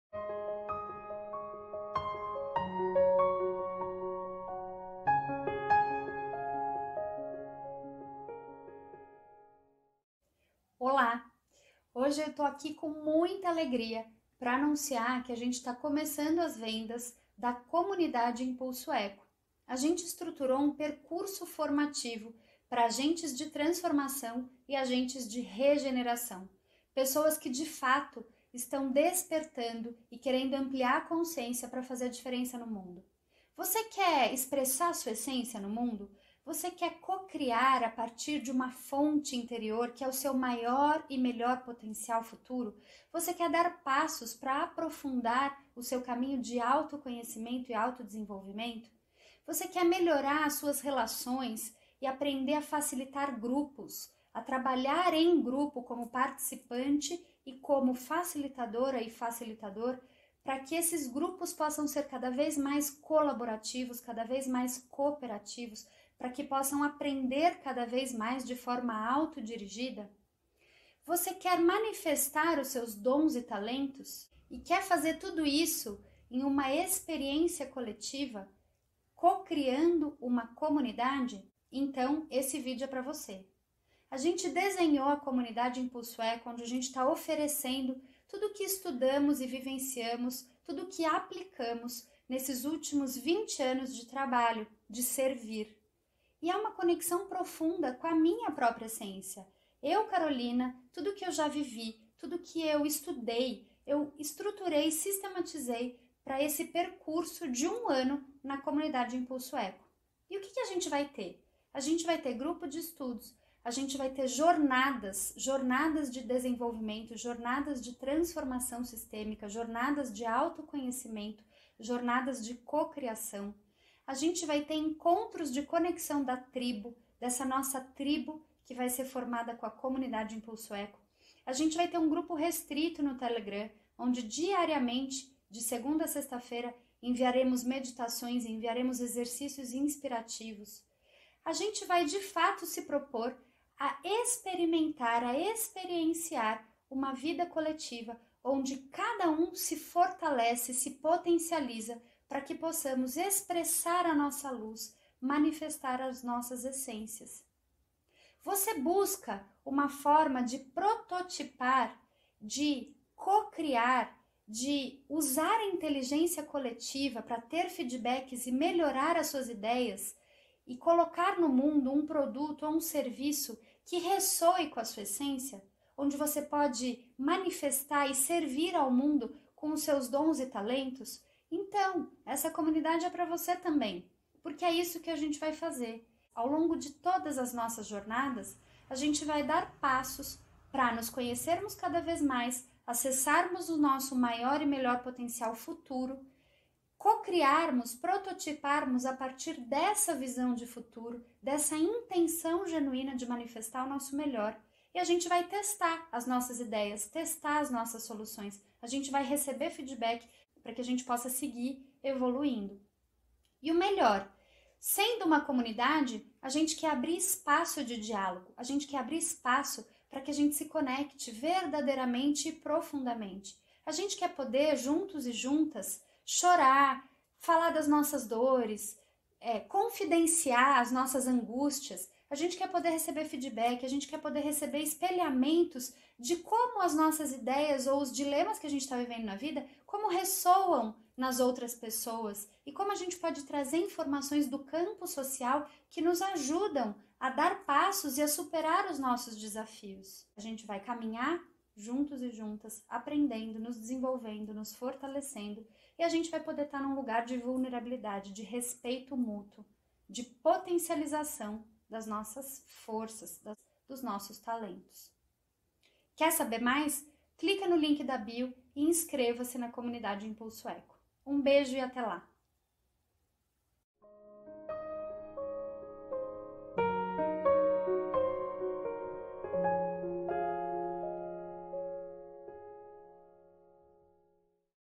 Comunidade impulso eco | inscrições abertas
Comunidade impulso eco: início em 24 de junho!

- Quer dar novos passos em seu caminho de autoconhecimento?
- Quer cultivar o solo para que a colaboração, a cooperação e a conexão profunda seja palco de novas parcerias e cocriações?
- Quer melhorar as suas habilidades como participante e facilitador(a) de grupos?
- Quer vivenciar jornadas baseadas em modernas tecnologias sociais como a antroposofia, a Teoria U e a psicologia social Pichoniana, para vivenciar e promover transformações sistêmicas baseadas na consciência?
- Quer participar de grupos de estudos para trocar ideias e fortalecer a sua bagagem como agente de transformação e de regeneração?
Vem fazer parte da comunidade impulso eco :)
Nossos encontros serão online e ao vivo pelo zoom, a partir de 24 de junho.
Escolha o seu plano e faça parte da nossa tribo! Link na bio.